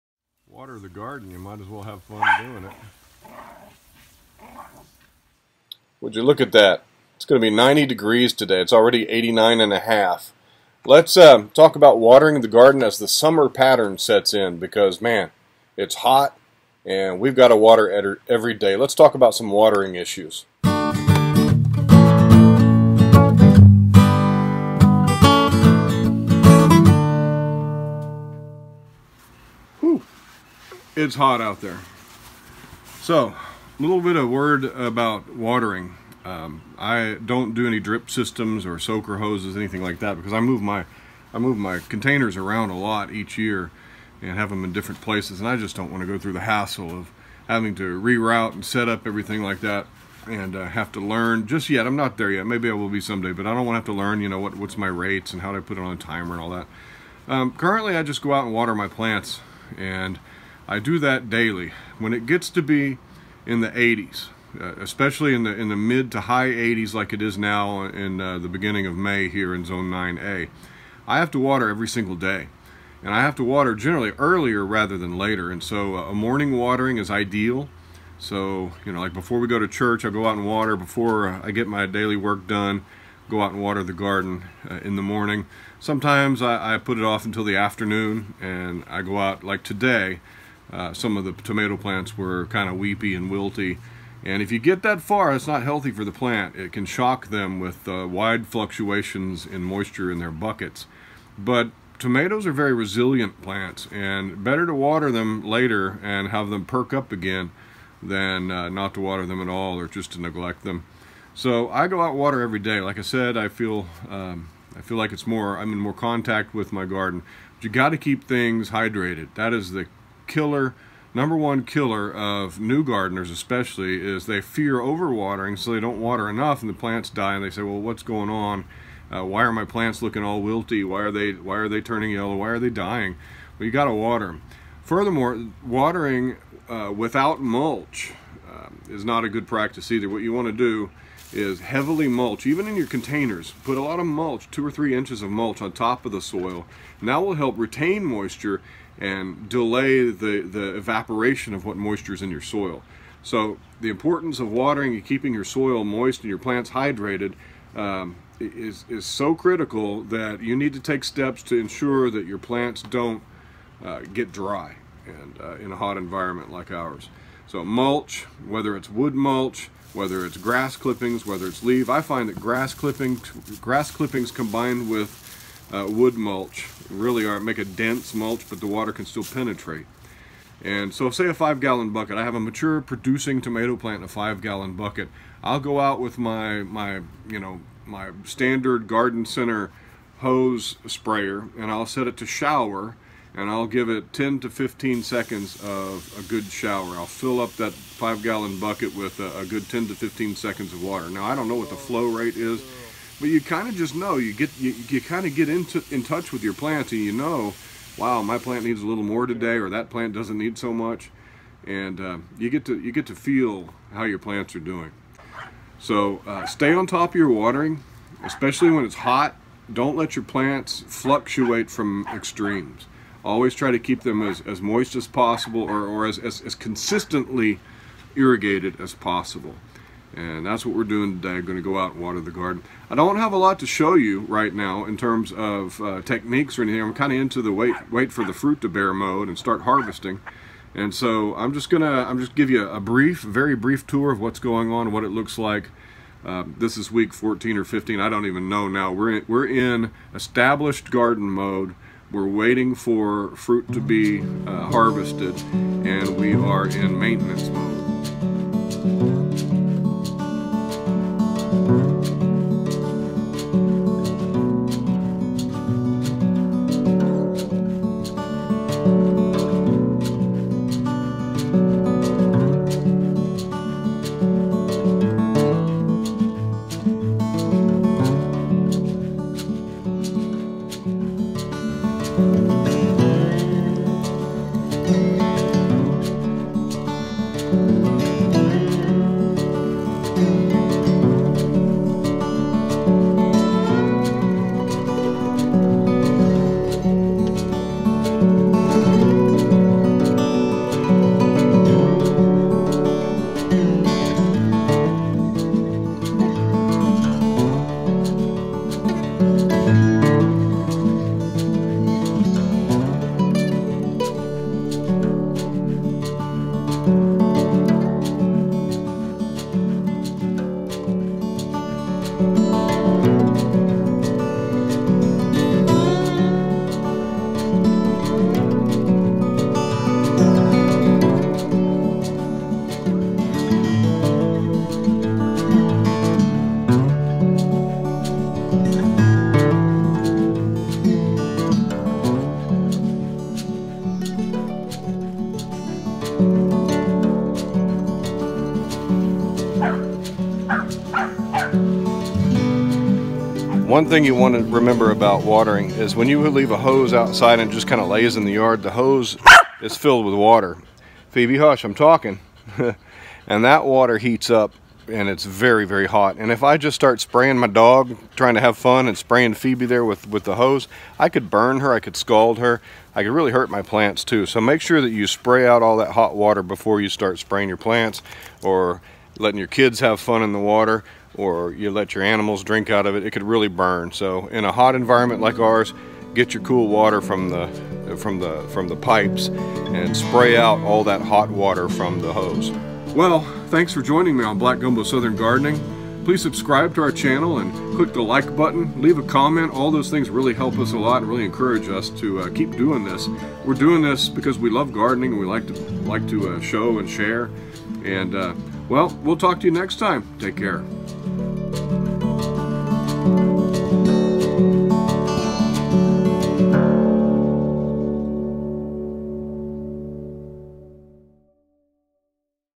Summer Pattern Watering || Black Gumbo
In our portion of Zone 9a along the Texas Gulf Coast, it is not uncommon for May to mark the beginning of summer-style heat. From May until September, we struggle with hot weather and constant hot southern winds off the Gulf that challenge almost all kinds of gardening. To keep a garden maintained through fruiting is our goal at this point. Included here is a very brief garden tour and a safety warning about scalding hot garden hoses, please watch, please share.